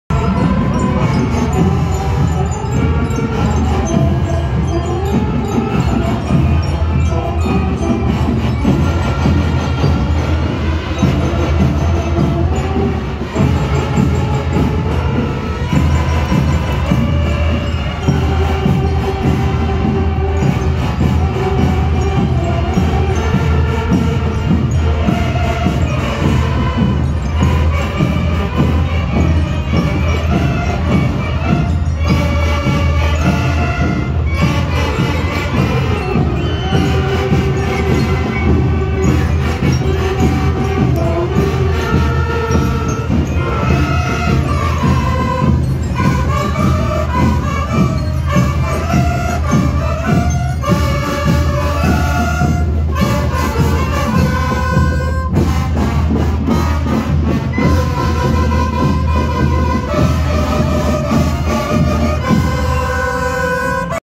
Desfile de Teas del 16 de Julio: Unidad Educativa Luis Espinal Camps
La Banda de la Unidad Educativa Luis Espinal Camps  en el Desfile de Teas del 16 de Julio en La Paz